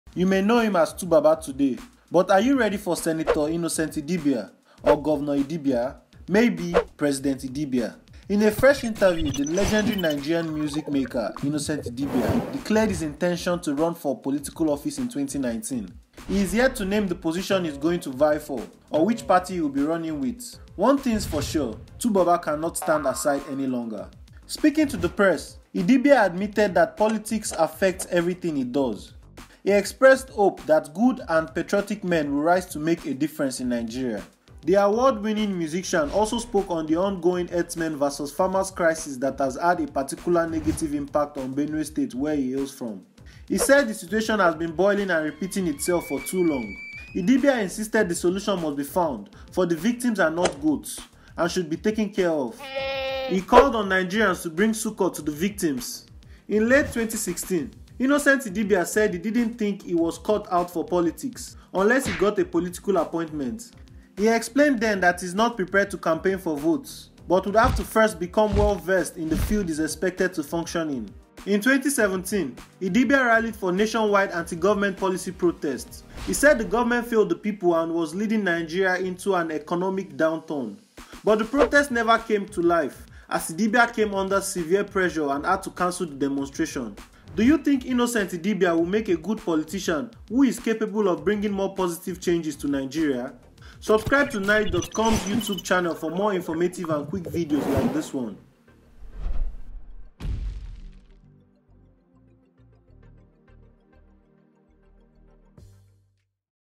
What if 2Baba Becomes President? Legit TV
In a fresh interview, the legendary Nigerian music maker Innocent Idibia declared his intention to run for political office in 2019. He is yet to name the position he is going to vie for, or which party he’ll be running with.